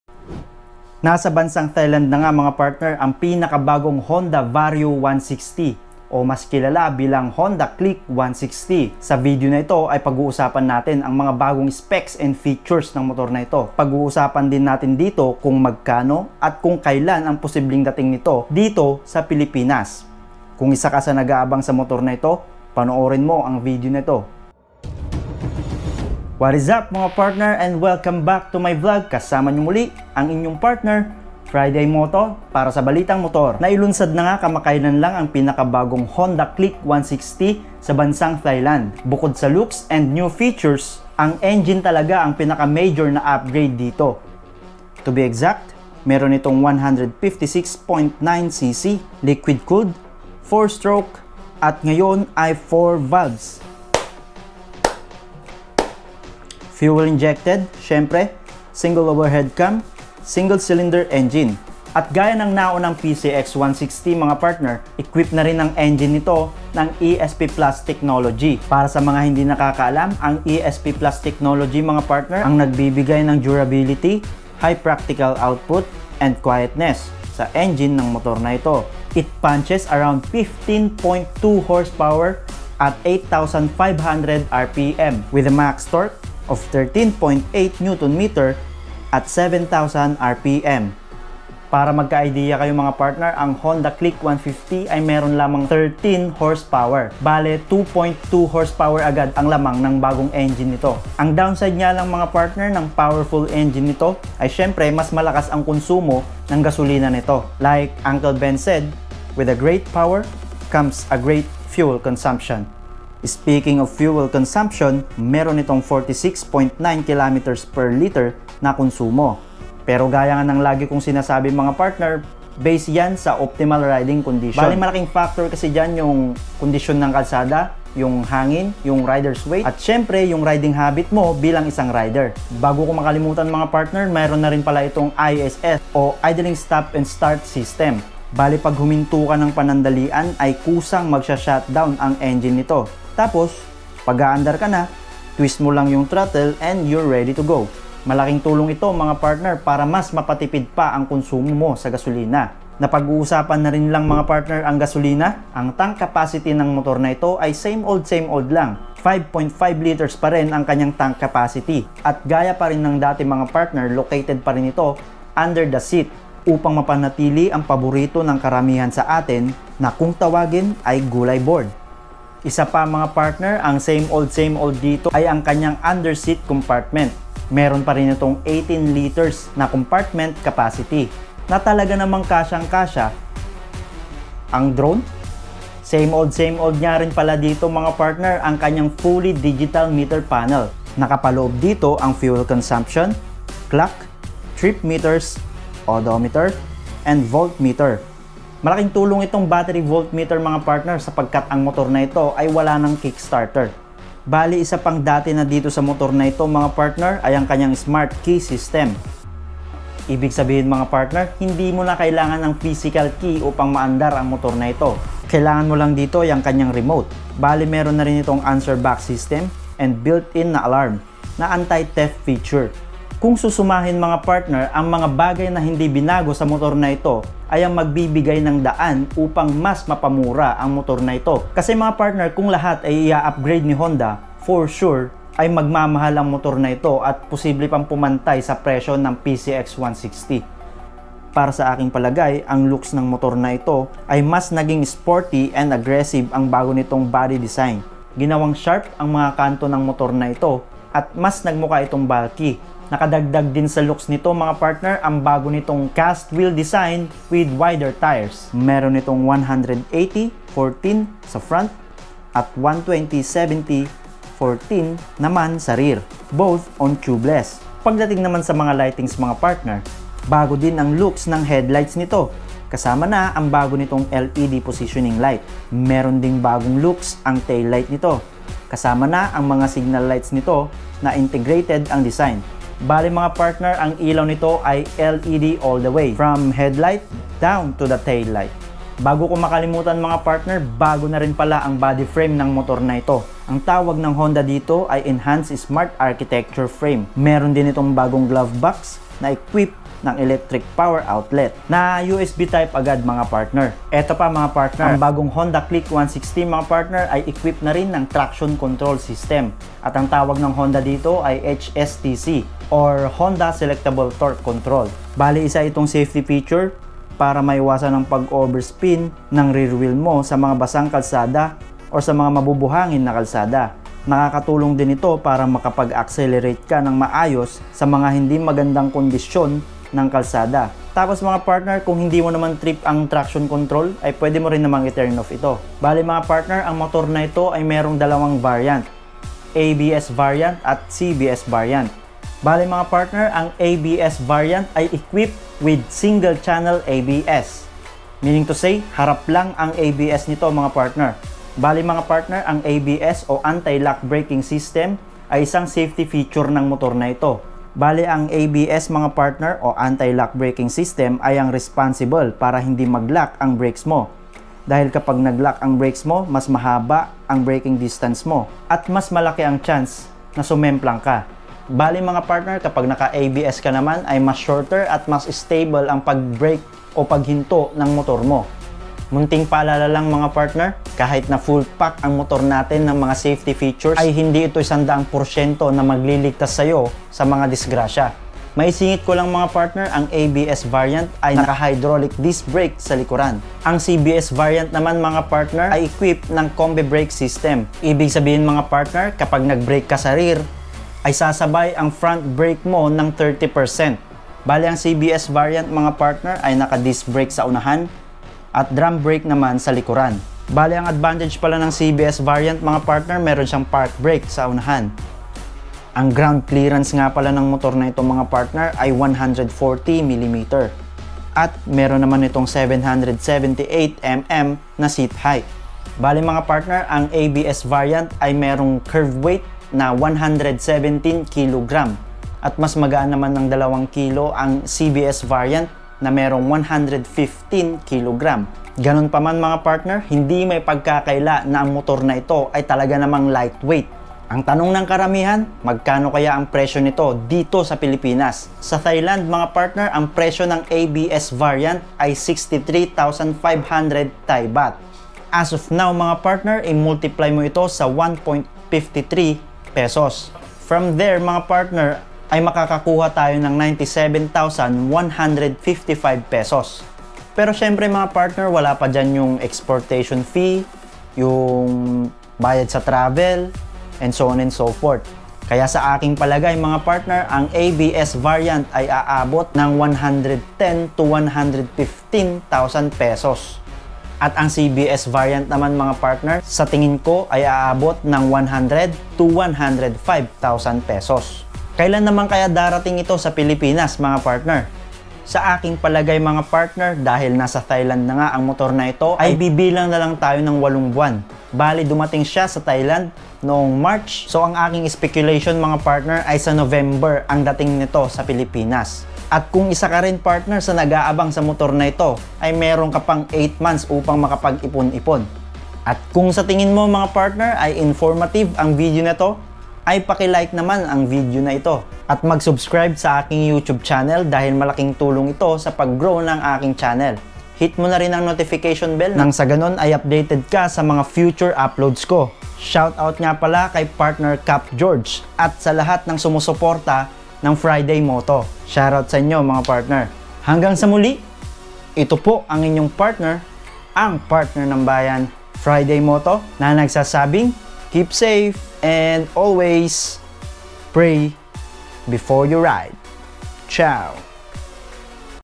ALL NEW HONDA CLICK 160 | SPECS AND FEATURE | VARIO 160
Sa video na ito ay paguusapan natin ang specs and features ng motor na ito.
Tatalakayin din natin kung magkano ang pusibling presyo nito dito sa Pinas.
Paguusapan din natin kung kailan ang posibling dating ng motor na ito dito sa bansa.